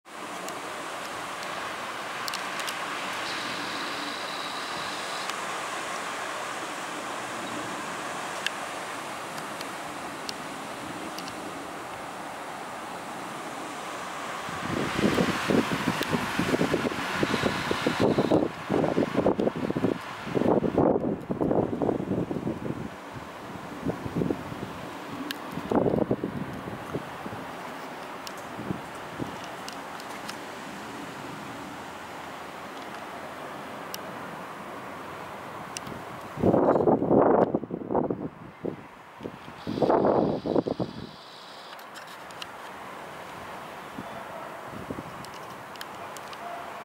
APOGEUM PORYWÓW WIATRU- NIŻ WTÓRNY HERWART
Silne porywy wiatru związane z niżem wtórnym Herwart. Ciśnienie w środku niżu wyniosło około 970 hPa. Przyniosło duże ochłodzenie.